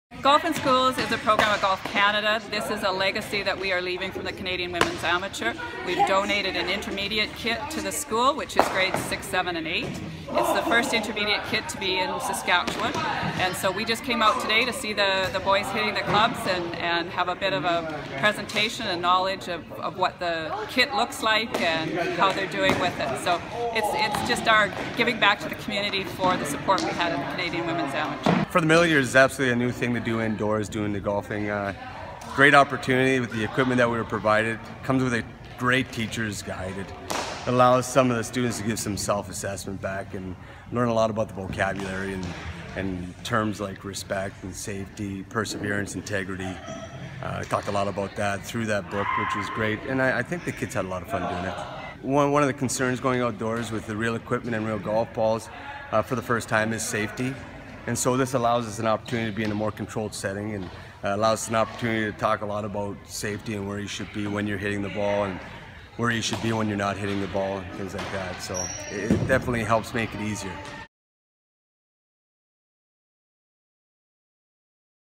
Golf Canada’s Golf in Schools program tees off in Clavet school
Through a legacy project from the 2015 Canadian Amateur women’s golf championship at Saskatoon’s Riverside Country Club, a Golf in Schools program has been introduced to middle-school students in Clavet.